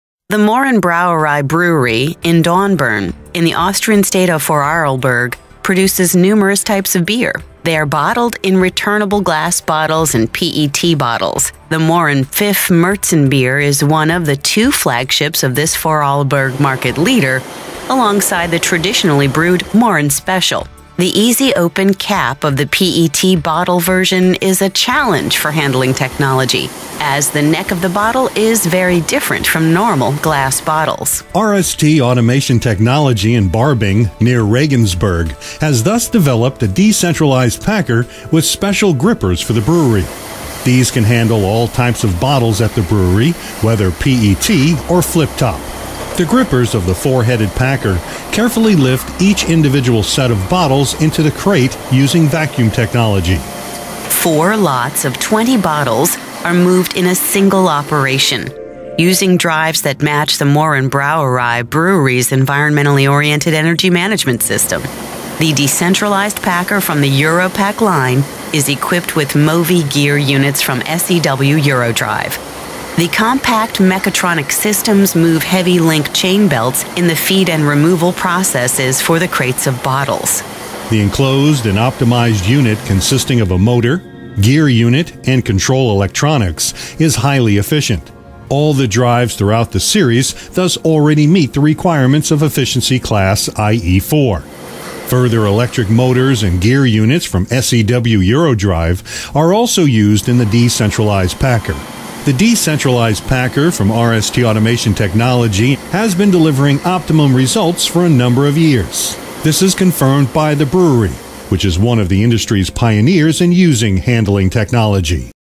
Decentralized packer with MOVIGEAR® | Brewery Mohrenbräu | SEW-EURODRIVE
Four lots of 20 bottles are moved in a single operation, using drives that match the Mohrenbrauerei brewery’s environmentally oriented energy management system. The decentralized packer from the “Europac” line is equipped with MOVIGEAR® units from SEW-EURODRIVE. The compact mechatronic systems move heavy link-chain belts in the feed and removal processes for the crates of bottles.